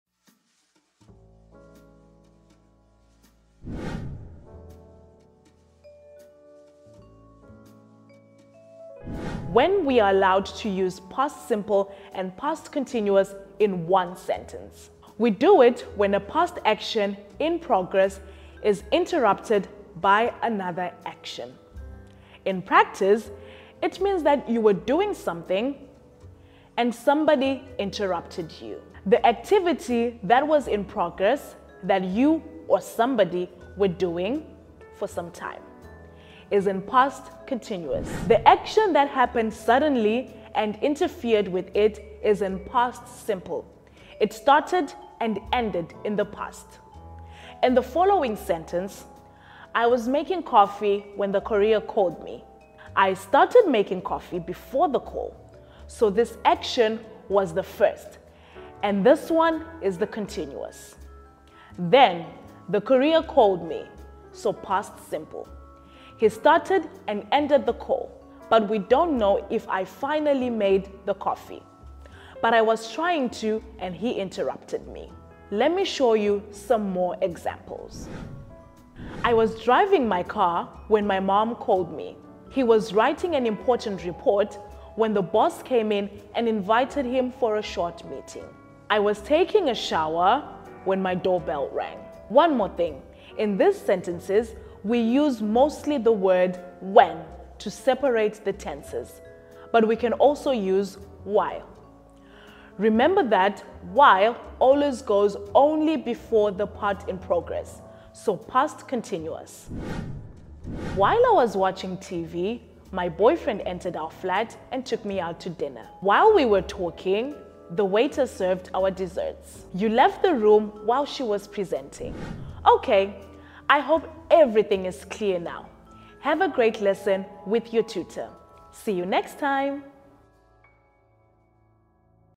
Naucz się używać Past Simple i Past Continuous W JEDNYM ZDANIU
Spis
0:00 - 1:17 Past Simple i Continuous w jednym zdaniu.
1:18 - 1:36 Przykłady zdań
1:37 - 1:50 Kiedy when, kiedy while?
1:51 - 2:13 Przykłady zdań